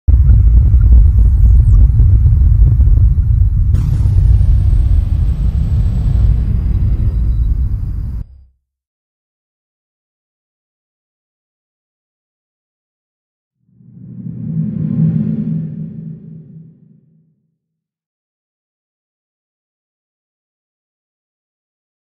２０２０明けましておめでとうございます！（happy new year !)
今年のお年賀動画は趣向をかえて日本の初日の出を宇宙から眺めました！（W）

ツール
after effects
video copilot. orb（freeのプラグインです）